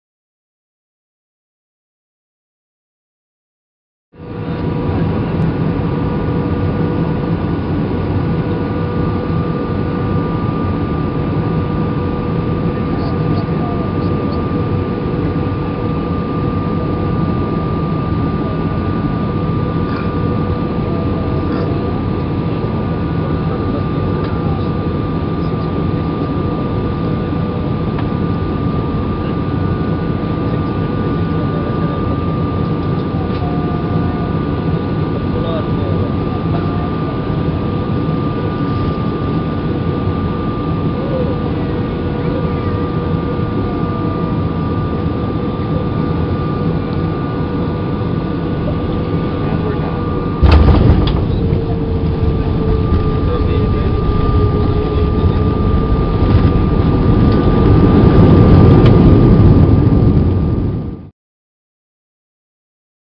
Landing at Mactan-Cebu International Airport
Took this with a Cybershot digital camera as my airplane from Manila coming home was landing at Mactan-Cebu International Airport. There was major turbulence on the entire trip! There was minor turbulence on our final approach and the pilot didn't do very well during landing. I just thought this footage would be a good memory to keep.